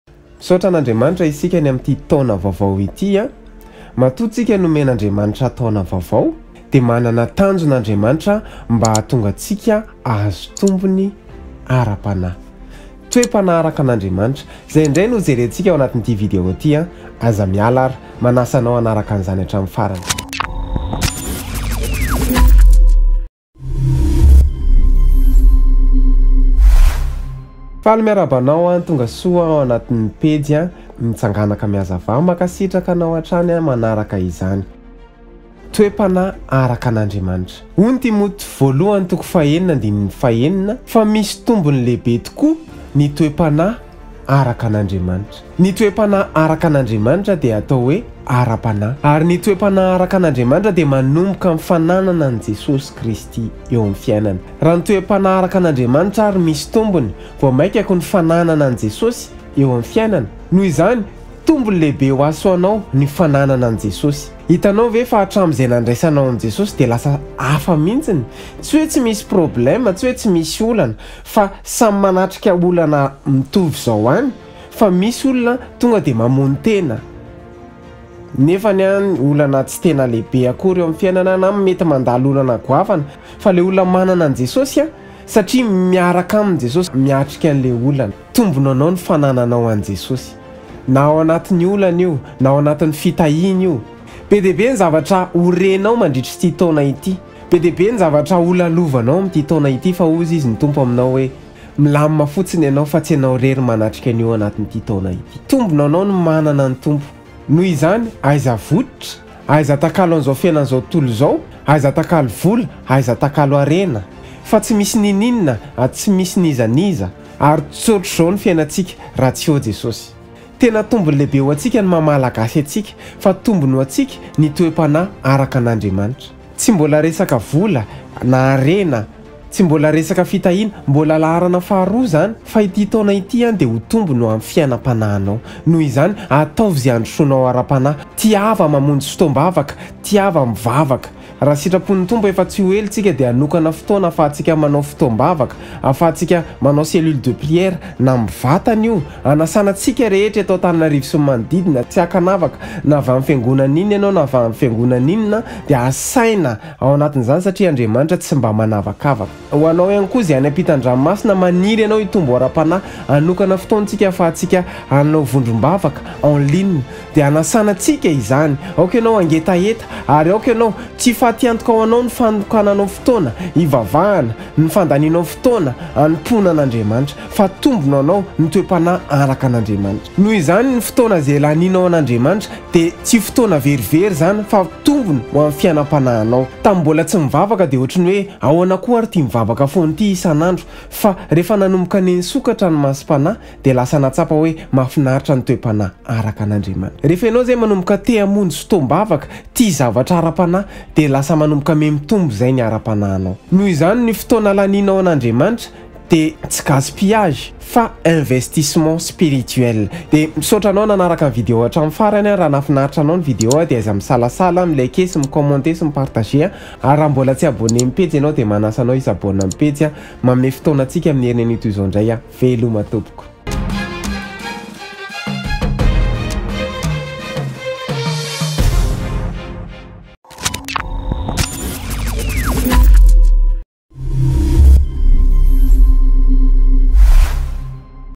TOE-PANAHY ARAKA AN'ANDRIAMANITRA😇
🤩Matoa isika nomen' Andriamanitra taona vaovao dia manana Tanjona Izy mba hatonga antsika hahazo tombony ara-panahy
🤩Manasa antsika hanaraka hatramin'ny farany
🤩Eo amin'ny commentaires ny liens Youtube